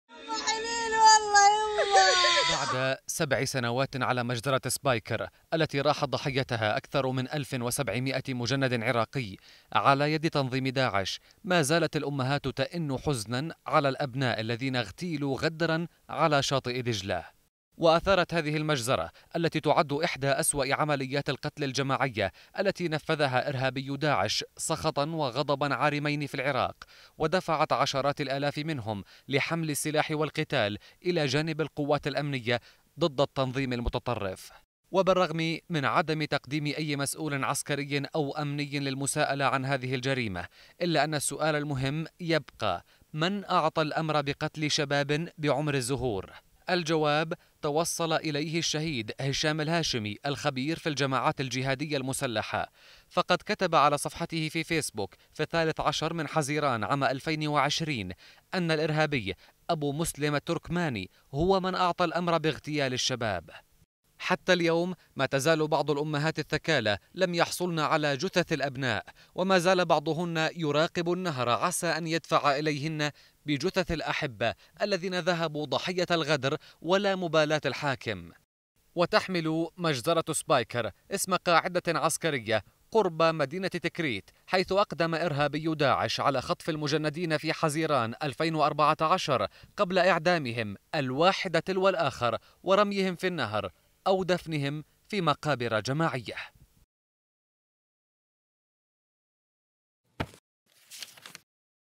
مجــزرة سبايكر لم تنتهي الى الآن ! | تقرير
مازال جرحُ مجزرة العصر سبايكر ينزف دماً مع انين ِ الأمهات الثكالى، اللواتي يبحث بعضُهن حتى الان عن جــثــ ـثِ الأحبة.
تقرير - غرفة الاخبار
قراءة - محمد شعفوط